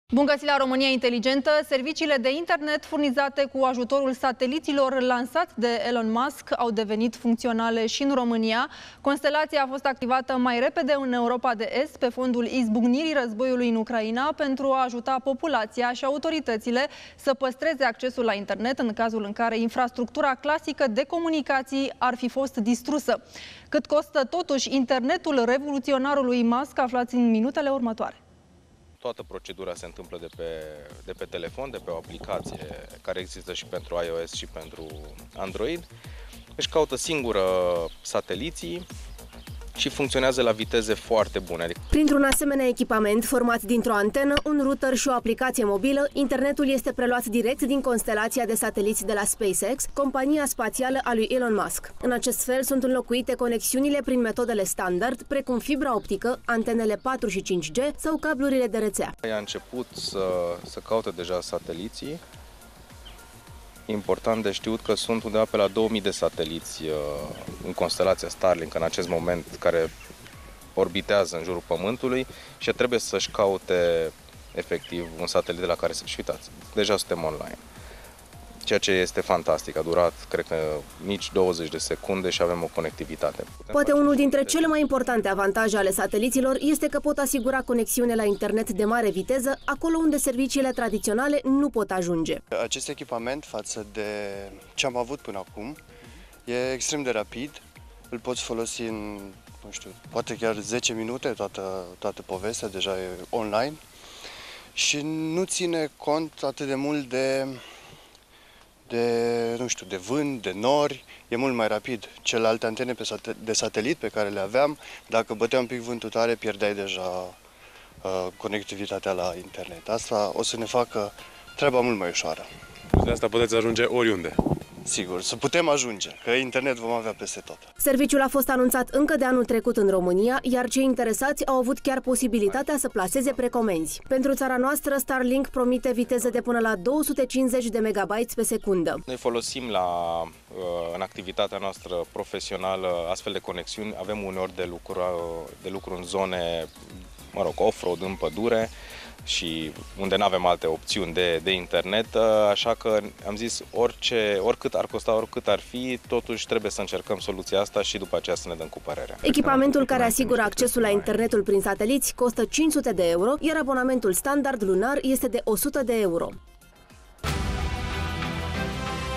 Internetul lui Elon Musk a fost activat în România. cât costă un abonament la Starlink
Internetul lui Elon Musk a fost activat în România. Ce viteză are și cât costă un abonament la Starlink
Constelația a fost activată mai repede în Europa de Est pe fondul izbucnirii războiului în Ucraina, pentru a ajuta populația și autoritățile să păstreze accesul la internet în cazul în care infrastructura clasică de comunicații ar fi fost distrusă.

Printr-un asemenea echipament, format dintr-o antenă, un router și o aplicație mobilă, internetul este preluat direct din constelația de sateliți de la Space X, compania spațială a lui Elon Musk.

În acest fel sunt înlocuite conexiunile prin metodele standard precum fibra optică, antenele 4G și 5G sau cablurile de rețea.
Poate unul dintre cele mai importante avantaje ale sateliților este că pot asigura conexiune la internet de mare viteză acolo unde serviciile tradiționale nu pot ajunge.

Serviciul a fost anunțat încă de anul trecut în România, iar cei interesați au avut chiar posibilitatea să plaseze precomenzi. Pentru țara noastră, Starlink promite viteze de până la 250 Mbps.

”Noi folosim în activitatea noastră profesională astfel de conexiuni. Avem uneori de lucru în zone offroad, în pădure, unde nu avem alte opțiuni de internet, așa că am zis că oricât ar costa trebuie să încercăm soluția asta și după aceea să ne dăm cu părerea”, spune Andrei Drăgan, antreprenor.

Echipamentul care asigură accesul la internetul prin sateliți costă 500 de euro, iar abonamentul standard lunar este de 100 de euro.